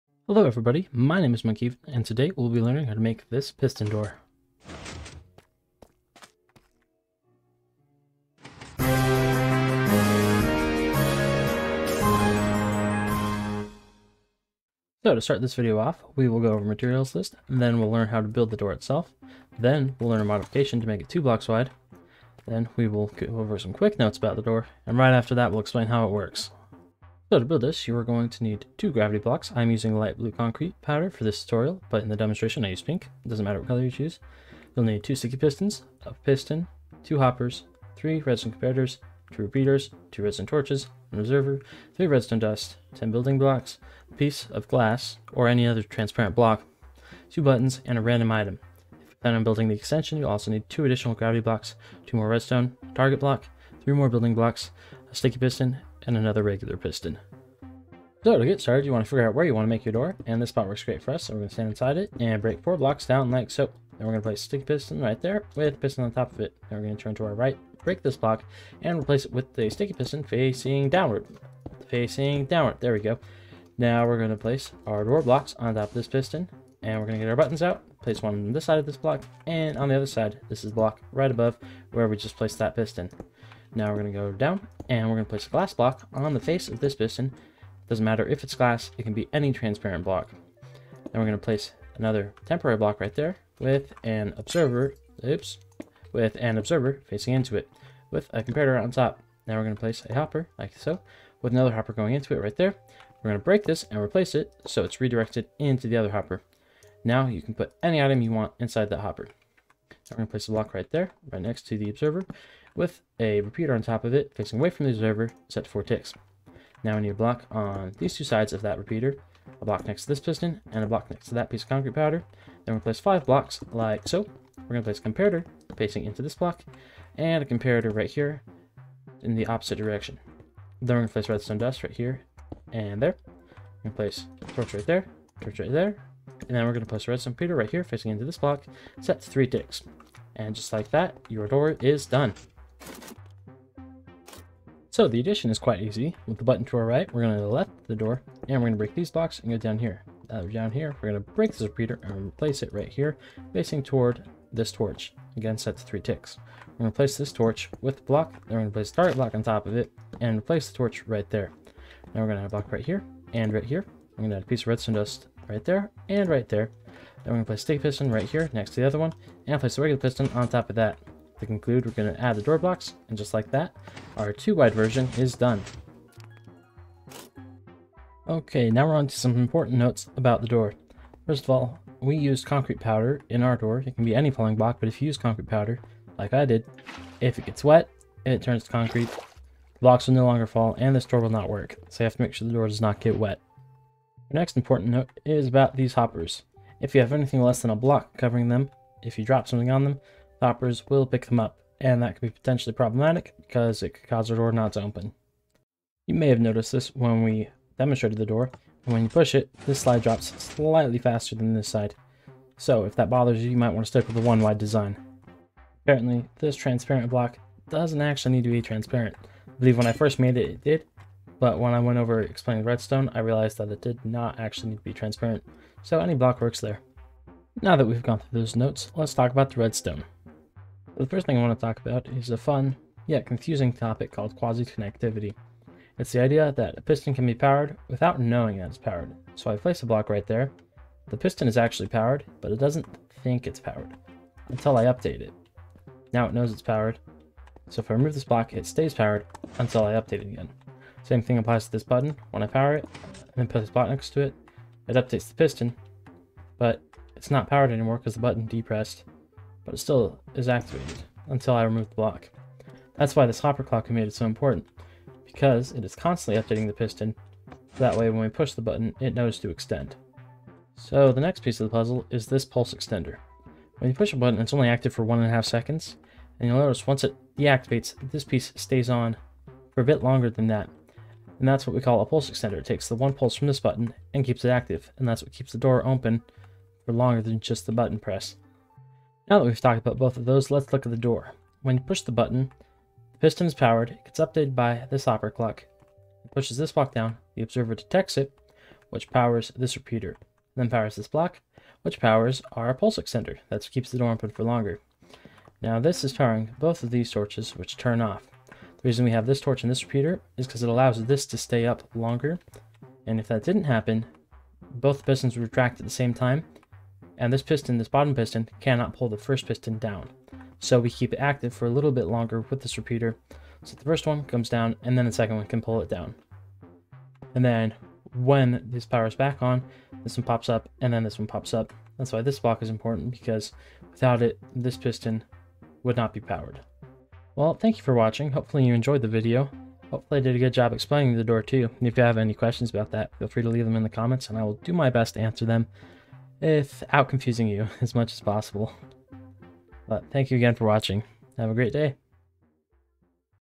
Concrete Powder Piston Door
Today we'll be making a simple piston door that uses gravity blocks and an interesting mechanic: quasi-connectivity.

Intro 0:00
Contents 0:14
Materials 0:26
Tutorial 0:57
2 block wide variation 2:24
Quick notes 2:57
How it works 3:47